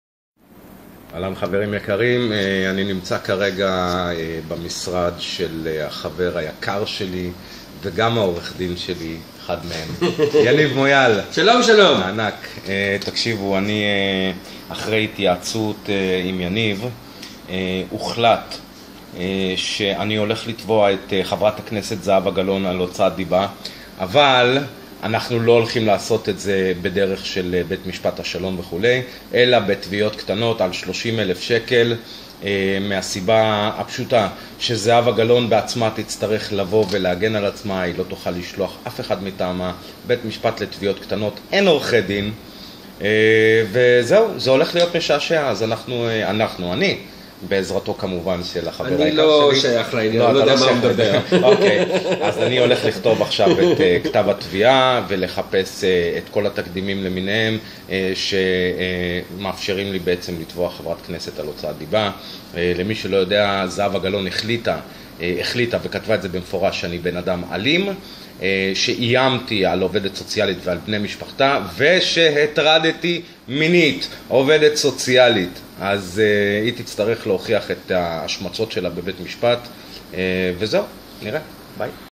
מכינים תביעה להוצאת דיבה נגד זהבה גלאון
אוגוסט 2016 - זהבה גלאון השתתפה בעליהום התקשורתי נגד בלוגרים פעילים חברתיים נגד פשעי משרד הרווחה, ואף הכפישה והשמיצה דברים שלא היו מעולם בדף הפייסבוק שלה נגד הבלוגרים. מוטי לייבל מגיש תביעת דיבה נגד זהבה